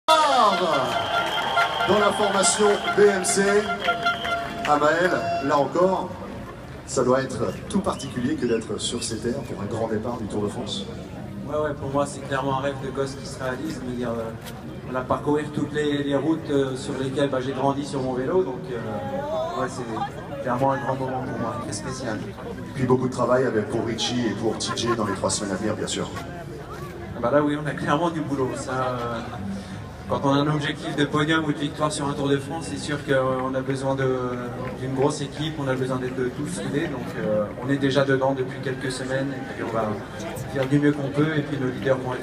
Présentation Tour de France 2016 - Amaël Moinard
Présentation des équipes du Tour de France 2016 à Sainte-Mère-Eglise (Manche). Jeudi 30 juin 2016.
Interview du cherbourgeois Amal Moinard (BMC).